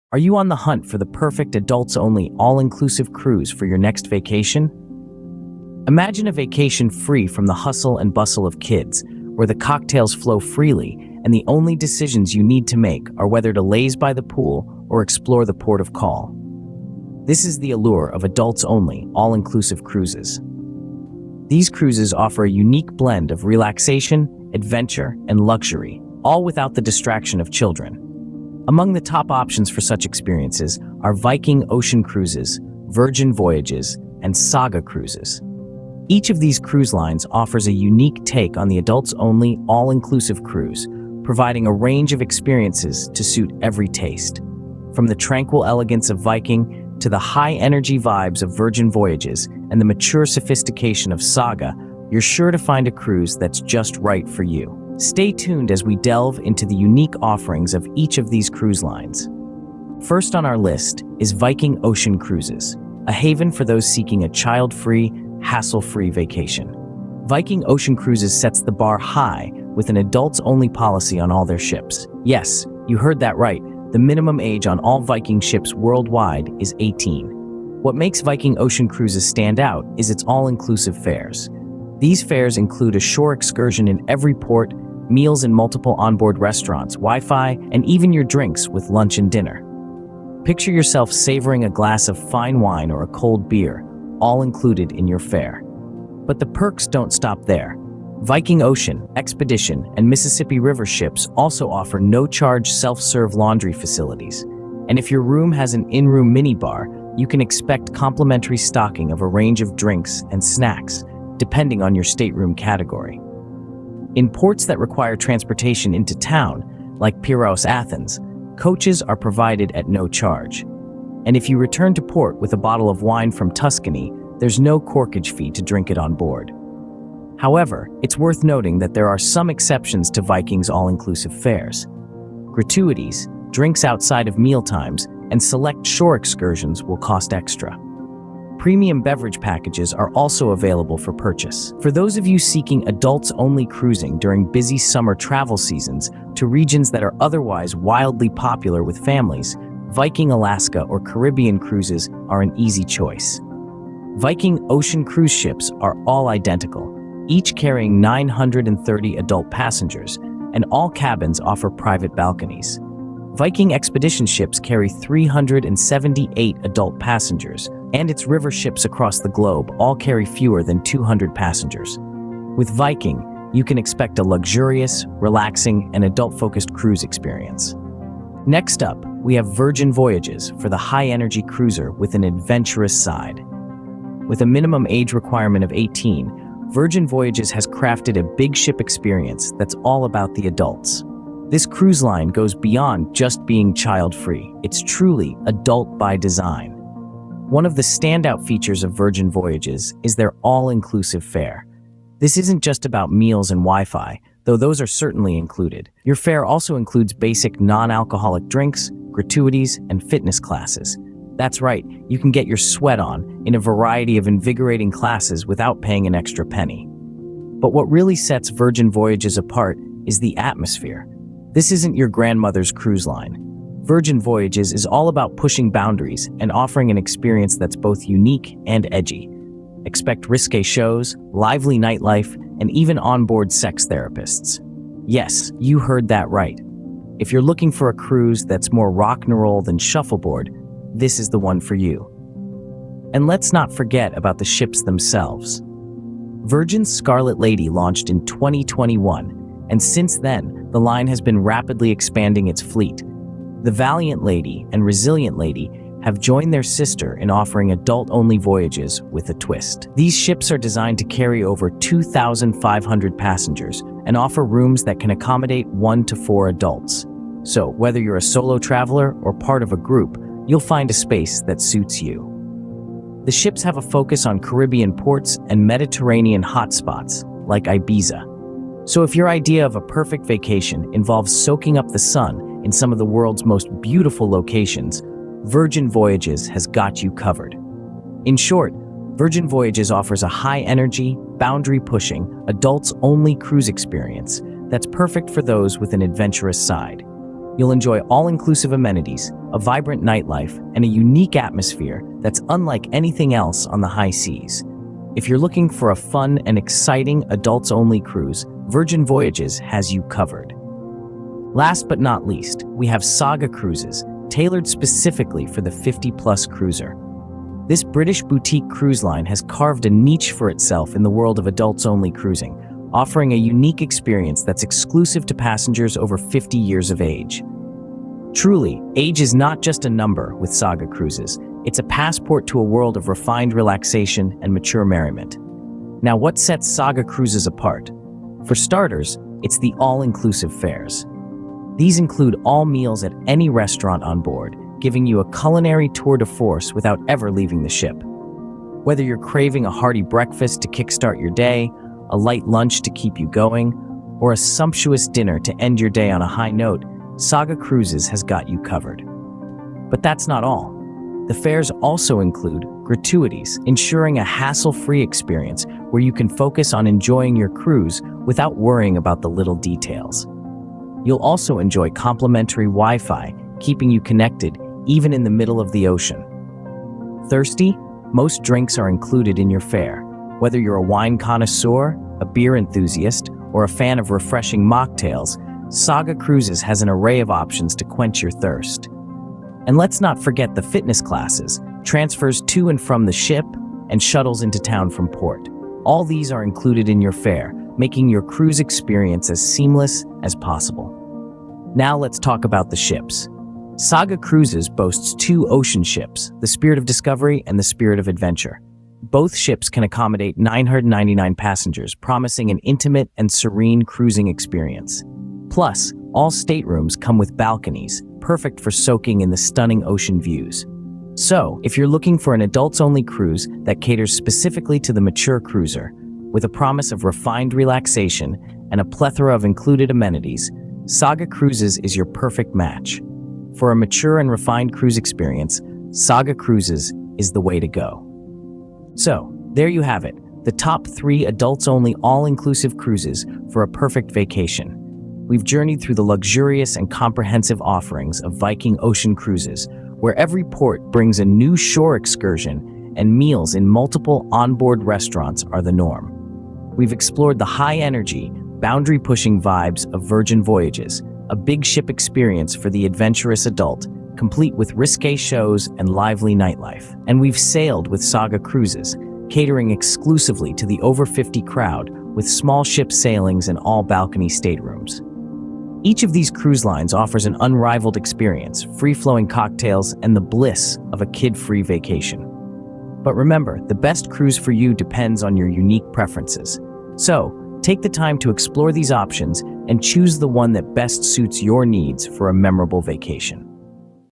Top 3 Adults-Only All-Inclusive Cruises: Unforgettable Escapes of 2024!
Embark on the ultimate adults-only getaway with these top 3 all-inclusive cruises of 2024! From luxurious amenities to unforgettable destinations, these cruises offer an unparalleled experience for those seeking relaxation and adventure without any age restrictions. Indulge in gourmet dining, sip on handcrafted cocktails, and unwind in lavish accommodations as you sail to exotic ports of call. Whether you're looking for romance, adventure, or simply some well-deserved pampering, these cruises have it all. Escape the ordinary and set sail on an unforgettable journey!

Top 3 Adults-Only All-Inclusive Cruises of 2024 by AIKONE's WORK ROOM
OUTLINE:
00:00:00 Introduction to Adult-Only All-Inclusive Cruises
00:00:59 Viking Ocean Cruises
00:02:58 Virgin Voyages
00:05:11 Saga Cruises
00:07:18 Conclusion

Cruise Ship Essentials